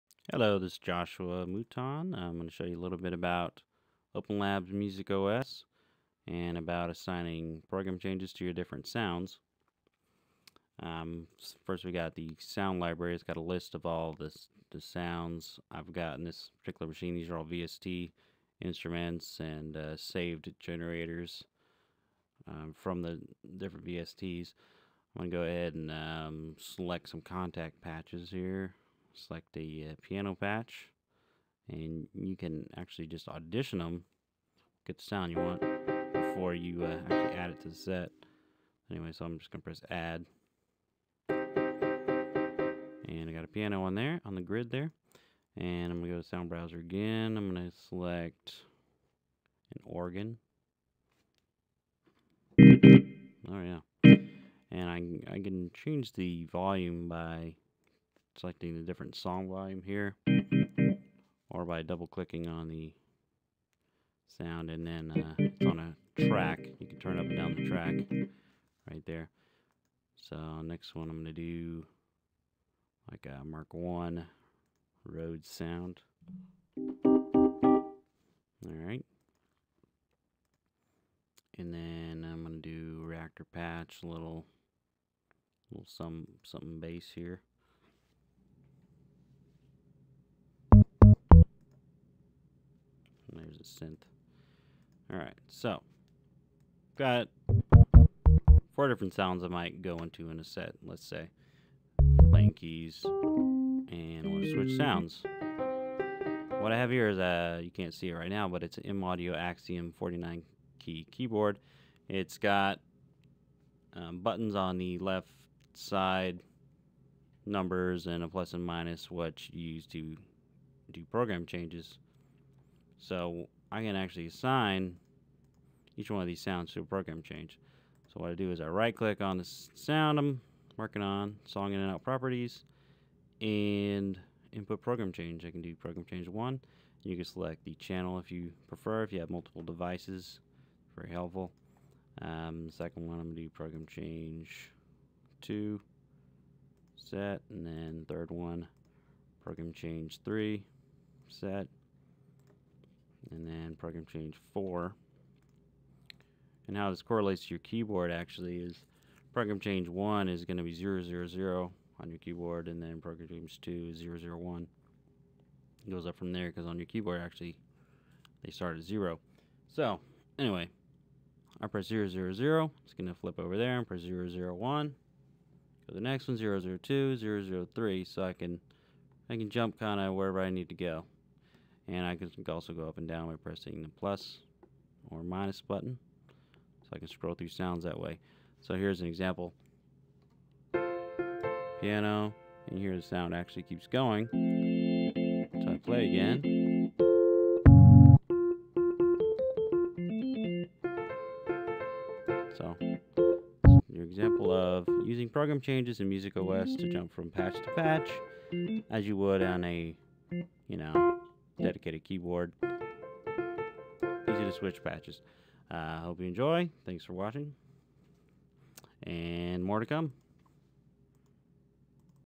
Open Labs Music OS Program Changes
You can assign program changes to your sounds and select them from your keyboard during a performance. Here is a short tutorial to show you the ropes using Open Labs Music OS 3.2.  Your vst instruments show up in the sound browser, you add them to the grid and assign program changes to them- simple!

I'm using an M Audio Axiom keyboard here.  Any keyboard with the ability to send program changes should work.